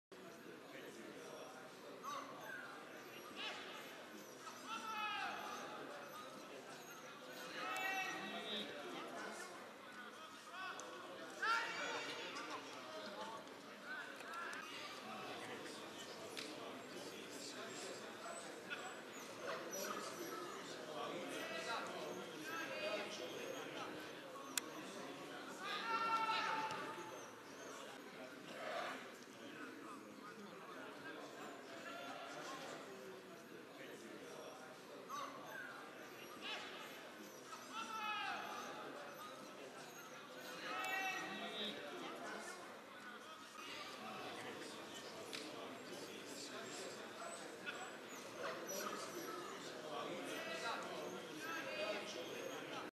Ardemagni e Camarà: l'abbraccio dopo la gag a 0825
Il famoso messaggio audio Whatsapp a Otto Channel archiviato con un abbraccio tra Matteo Ardemagni e Idrissa Camara